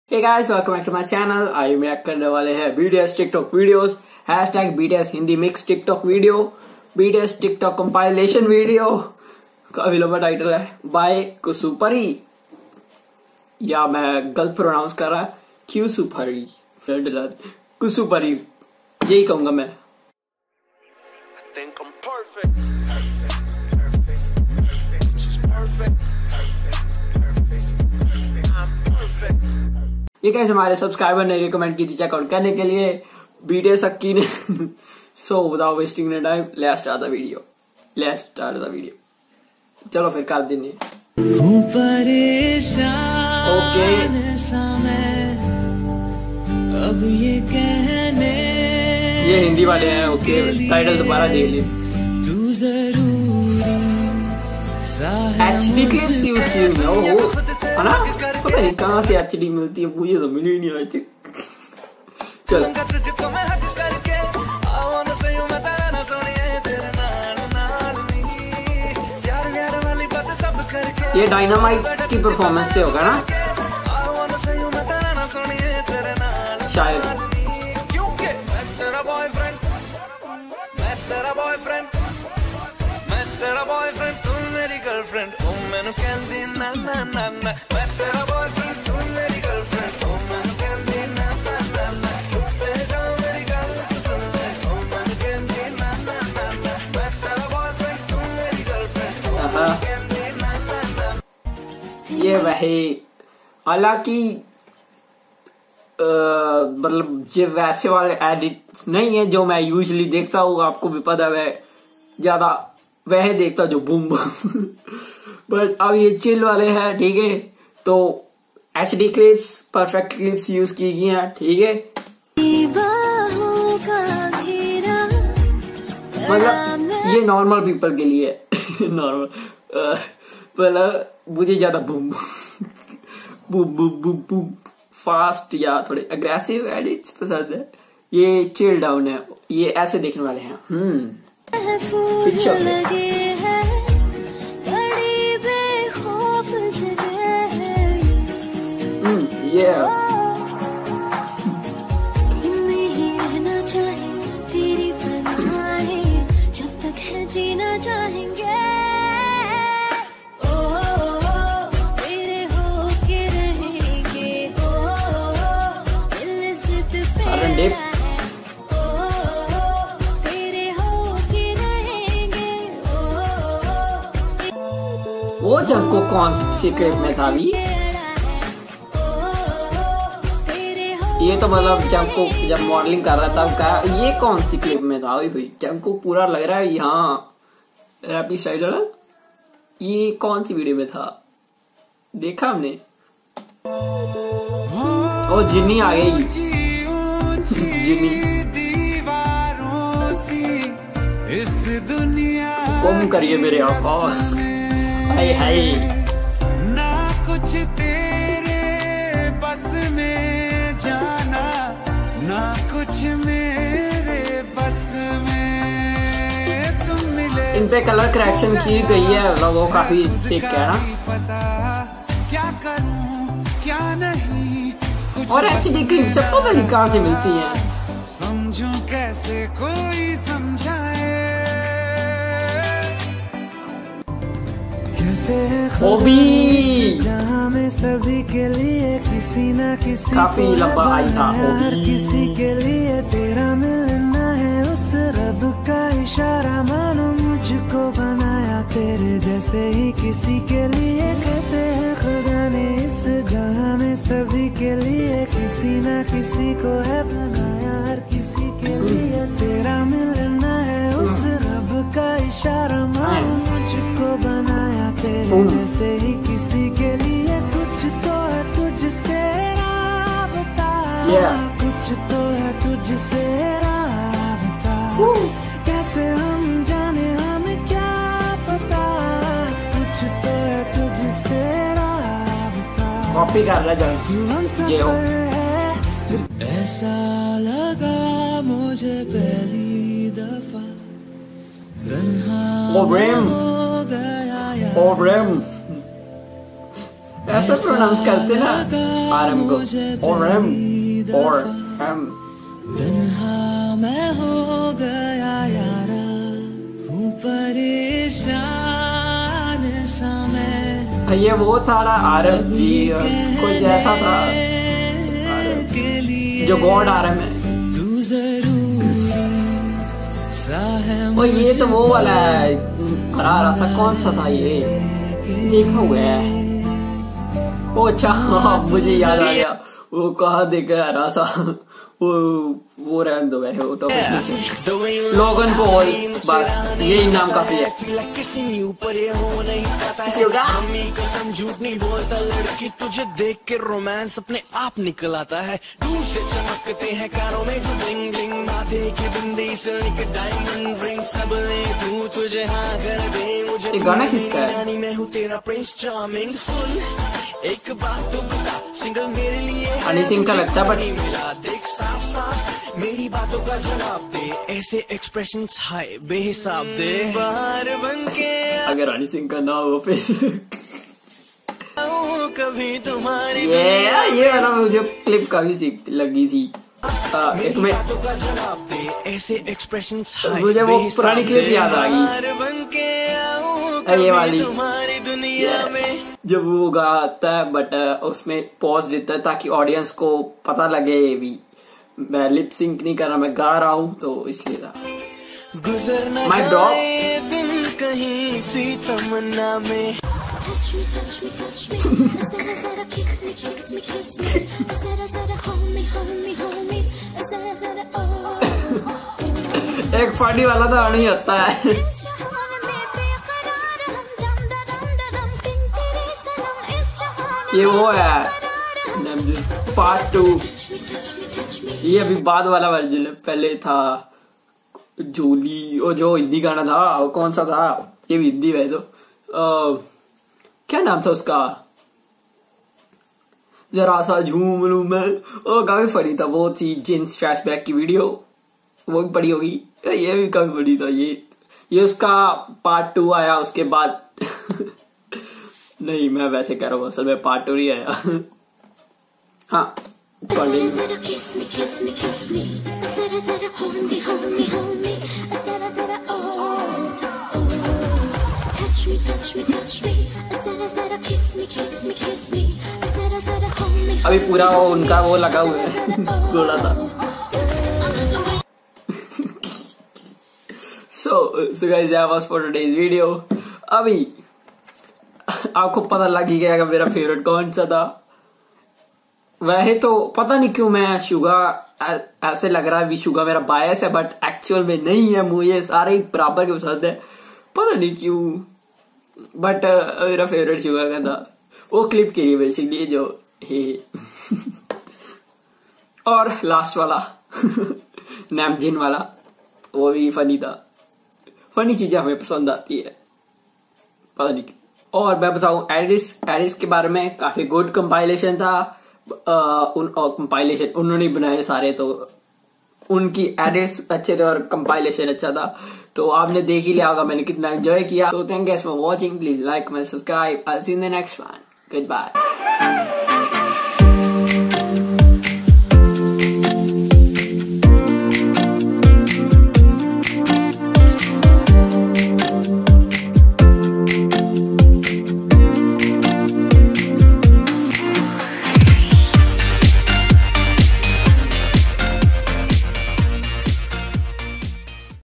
BTS TIKTOK HINDI VIDEO COMPILATION \ INDIAN REACTION
Hi Guys I Hope U liked This video
OriginaVideo - BTS Tik Tok Video.🔥Bts Hindi Mix TikTok video. 🔥 Bts tik tok compilation video. Kusu_Pari . channel